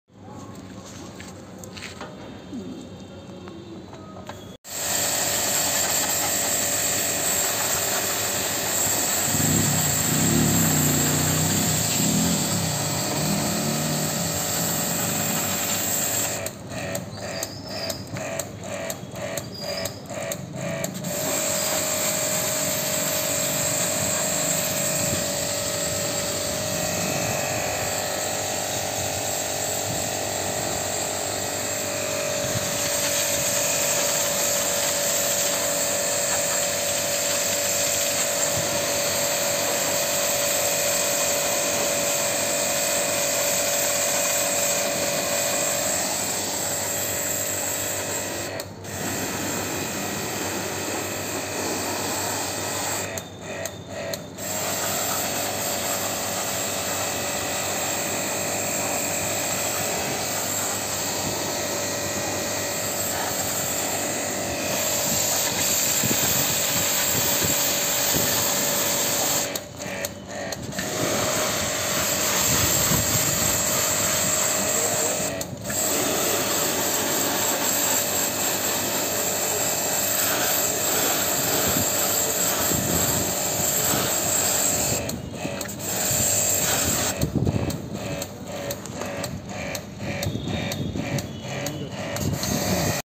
split AC blower service - wash with high pressure Jet pum
ac blower completely Cline Jet pum